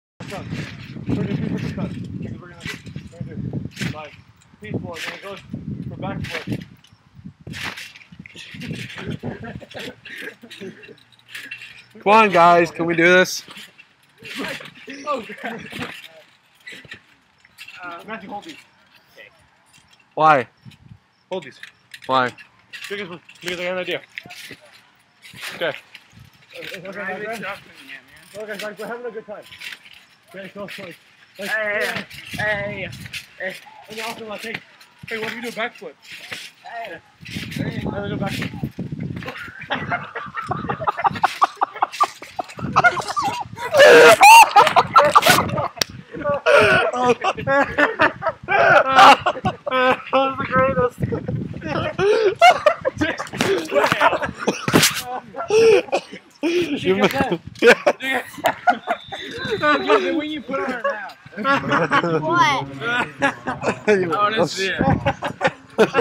BackFlip Fail!!
Austin tries to do a backflip but fails...

LOL